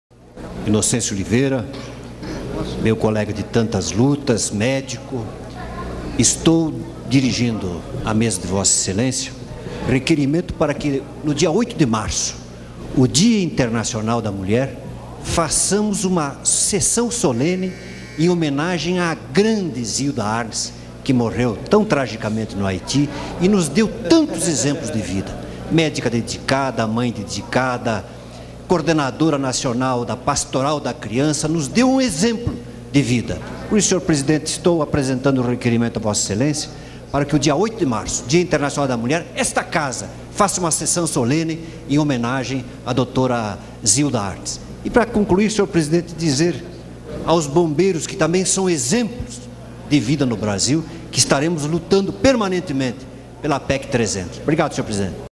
Deputado Alcenir Guerra envia pedido de sessão solene para homenagear Zilda Arns
Deputado Alcenir Guerra (DEM-PR), Comunica envio de requerimento para realização de sessão solene de homenagem ao dia internacional da mulher, comemorado em 3 de março. A intenção do deputado homenagear principalmente a sra. Zilda Arns, médica coordenadora internacional da Pastoral da Criança, morta em terremoto no Haiti, em 2 de janeiro deste ano. Por fim, declara apoio à votação da PEC 300.